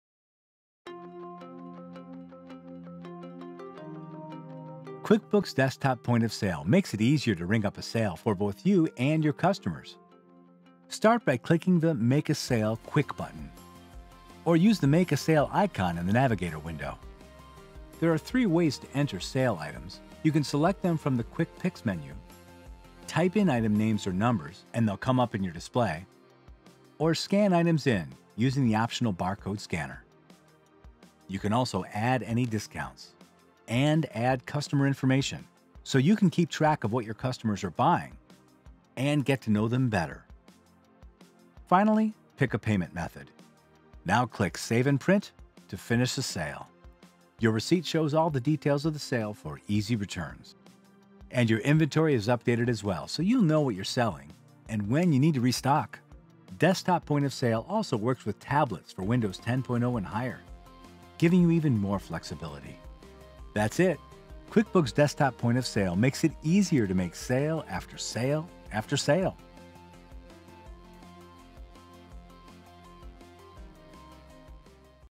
QuickBooks Desktop Point of Sale: How to Make a Sale
Making a sale in QuickBooks Point of Sale allows you to ring sales that you want to.  Accept credit and debit card payments that are integrated with your cash register and inventory.